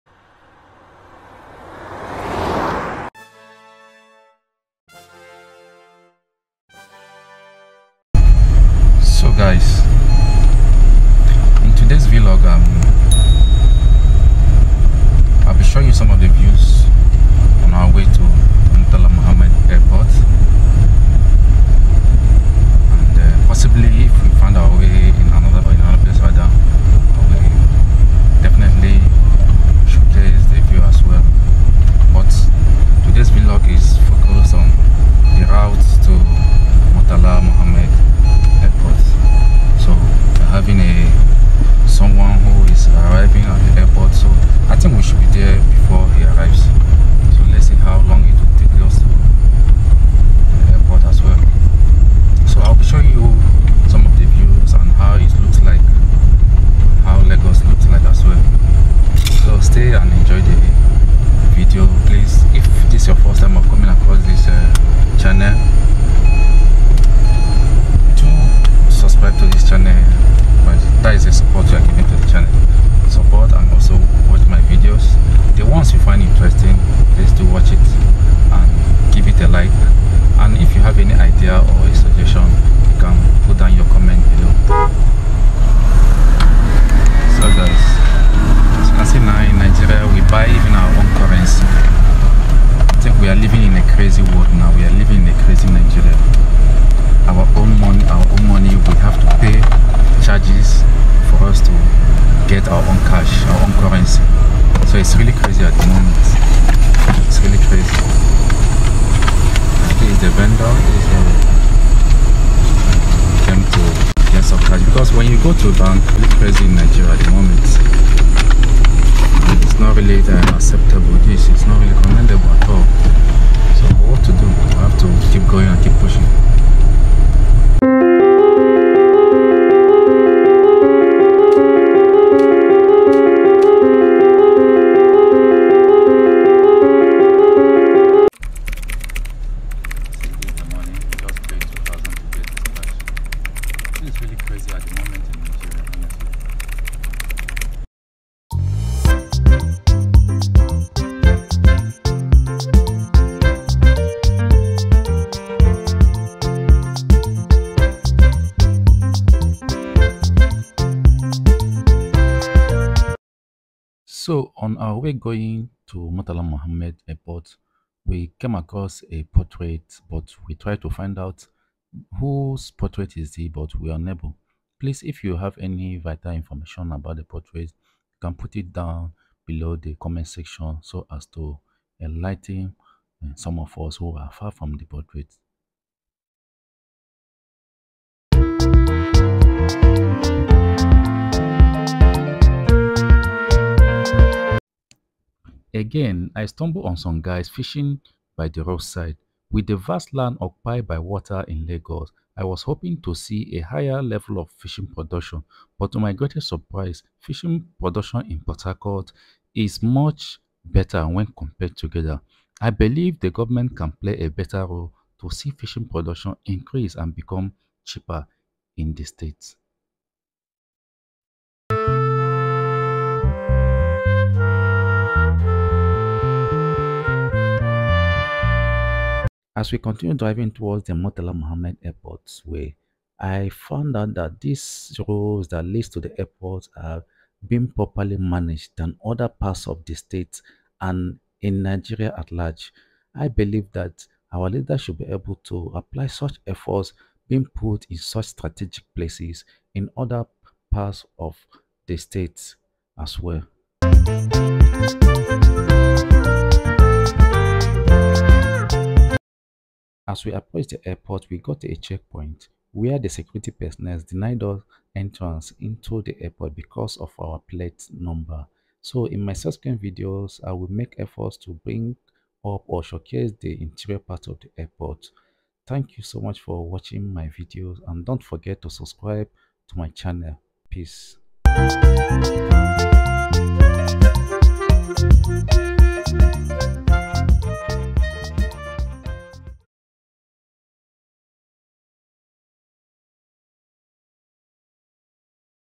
The airport people talked about
This video is to showcase the view to Lagos airport.

Do enjoy the video!